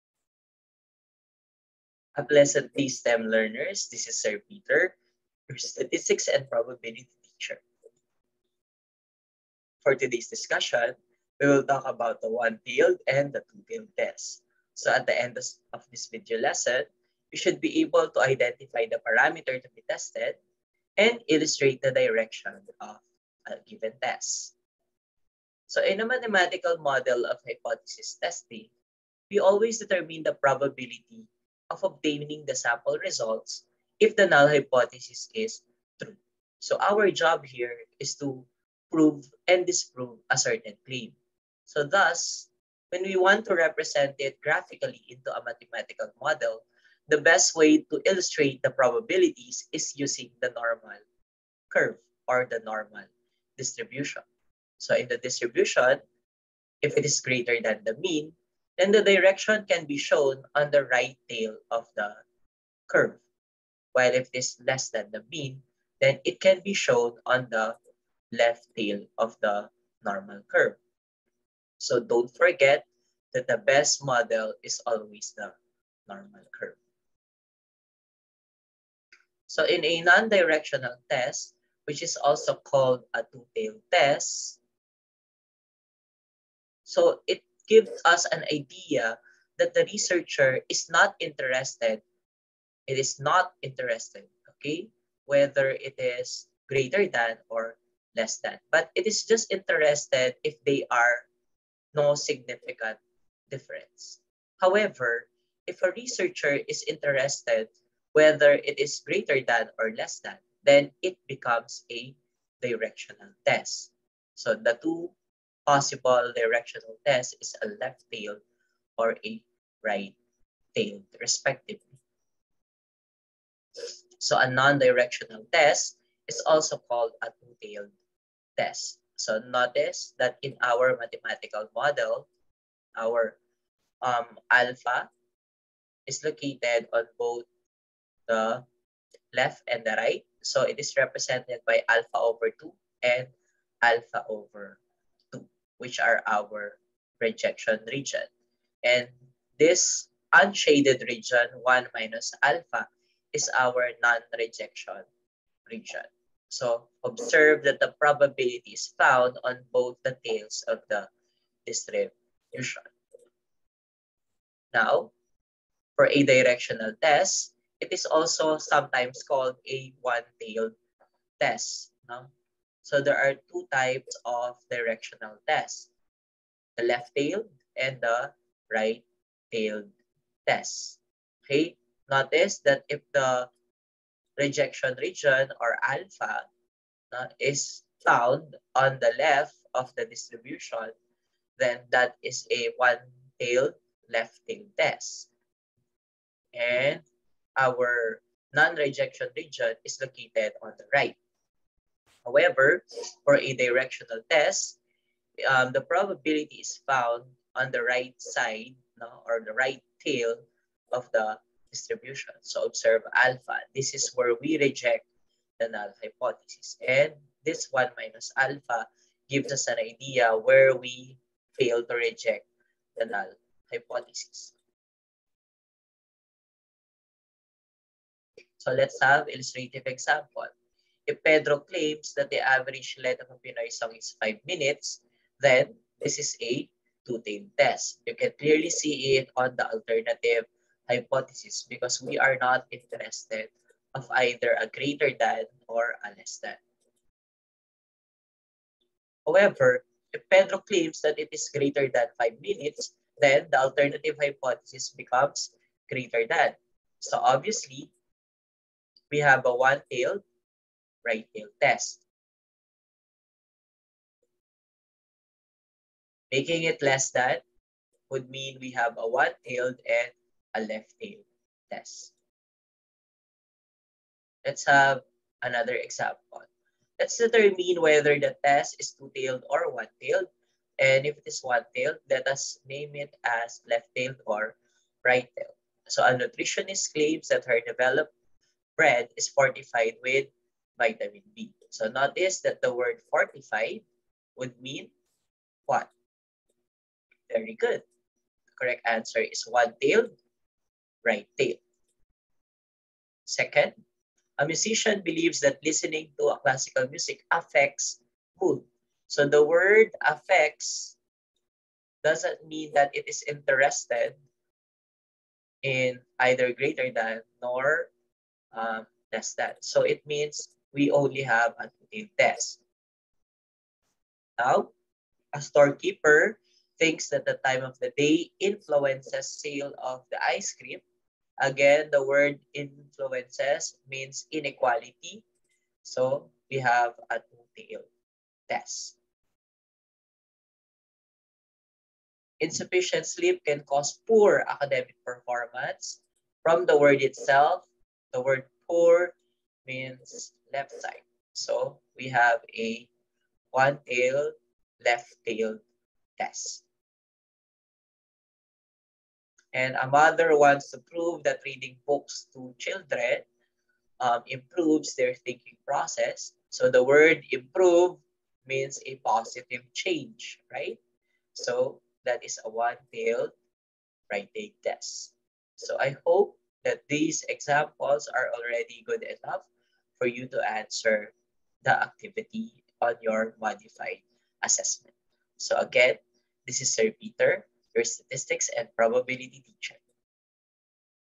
One-tailed and Two-tailed Test (Grade 11 Statistics and Probability)
At the end of this video lesson, you should be able to identify the parameter to be tested in the given problem and to illustrate the direction of the test.